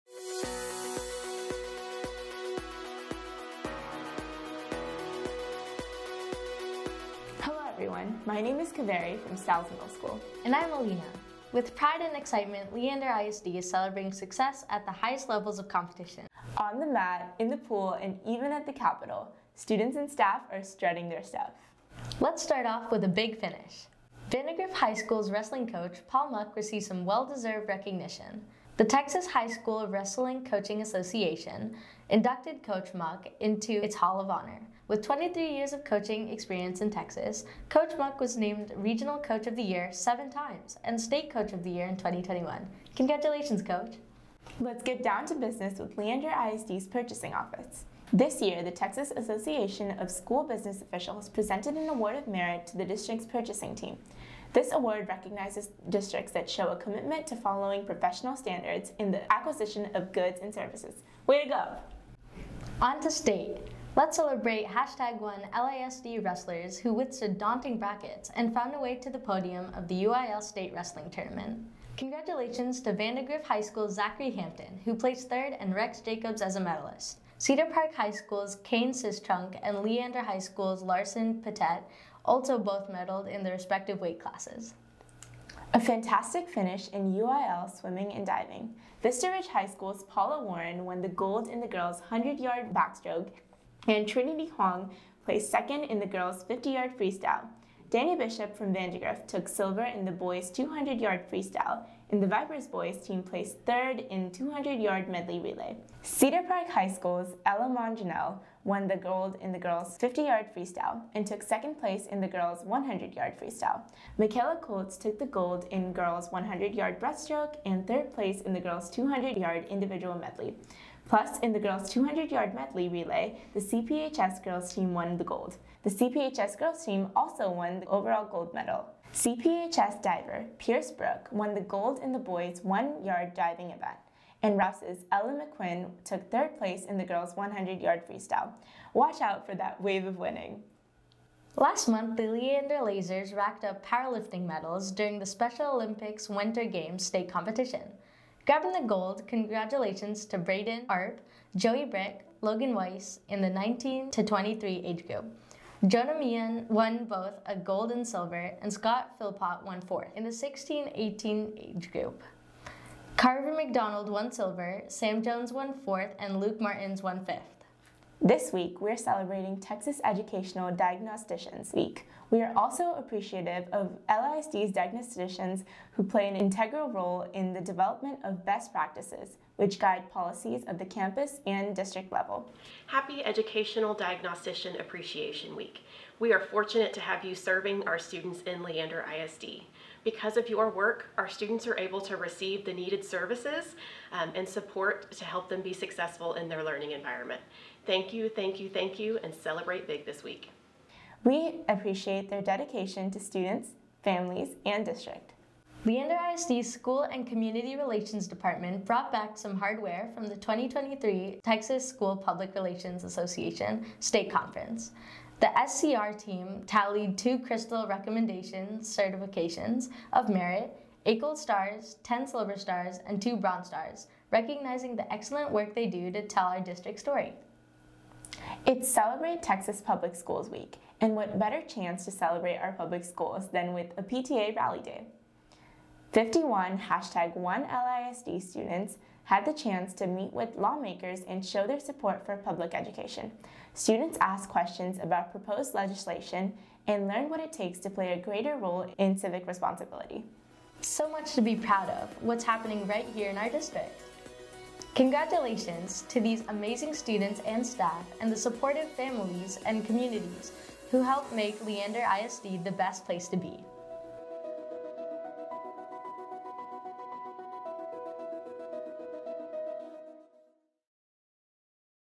Board Recognitions: March 9, 2023
During the March 9 Board meeting, Trustees celebrated the following groups and individuals:
– Vandegrift HS wrestling coach Paul Muck for being inducted into the Texas High School Wrestling Coaching Association (THSWCA) Hall of Honor
– The LISD Purchasing Department for receiving the Award of Merit by the Texas Association of School Business Officials (TASBO)
– LISD wrestlers, swimmers and divers who medaled at recent UIL state competitions
– Leander Lazer powerlifters who medaled at the state Special Olympics competition
– Texas Education Diagnosticians Week
– The LISD School & Community Relations Department for receiving awards from the Texas School Public Relations Association (TSPRA)
– Celebrate Texas Public Schools month, highlighted by the students' recent trip to the Texas Capitol during PTA Rally Day

Special thanks to Kaveri Mukherjee and Alina Sheikh of Stiles Middle School for anchoring these recognitions.